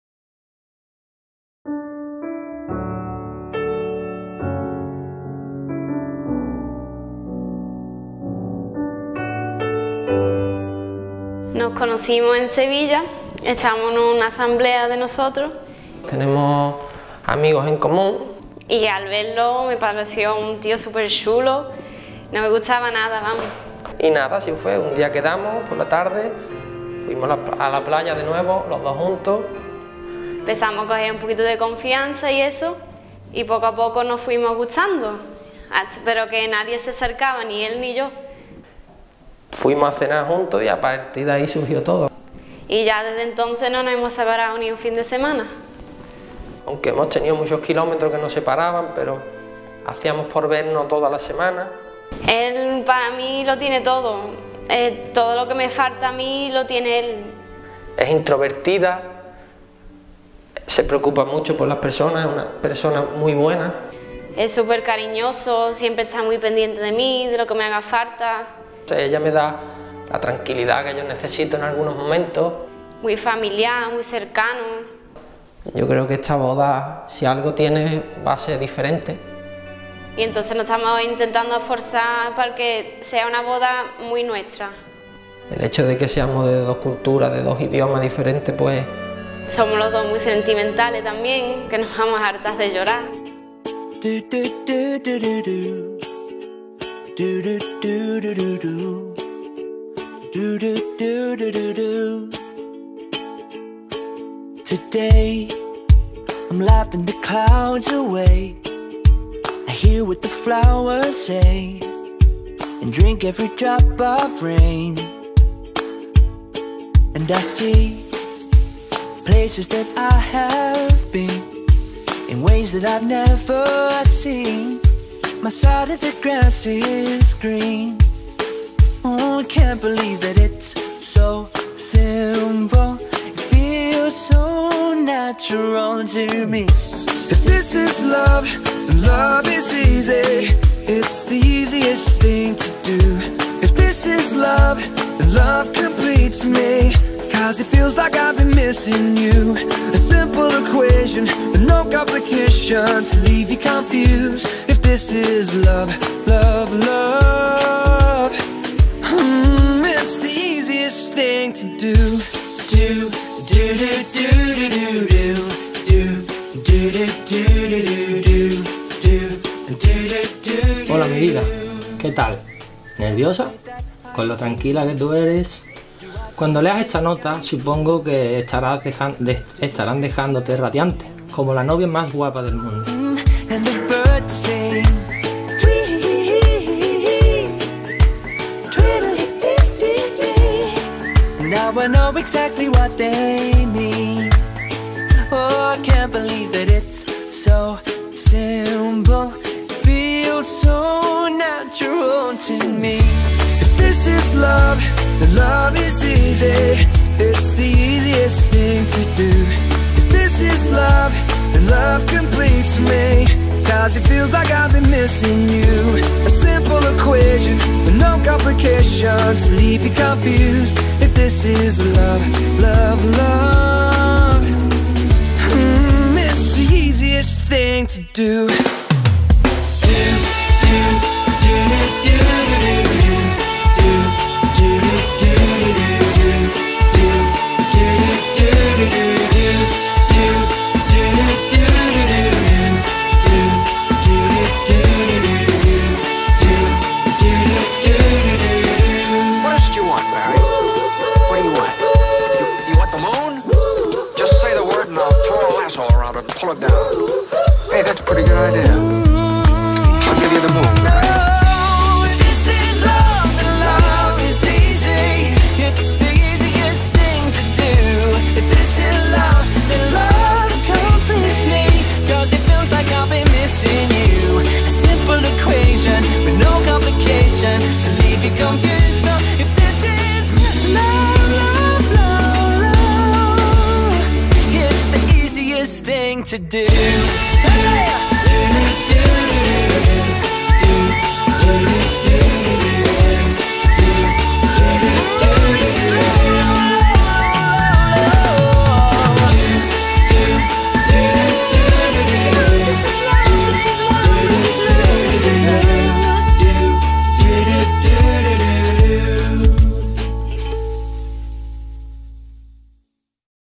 Video de boda Gibraltar. Wedding Video Gibraltar. Elí + Rhoda. Edición en directo
Video grabado y proyectado en la misma boda.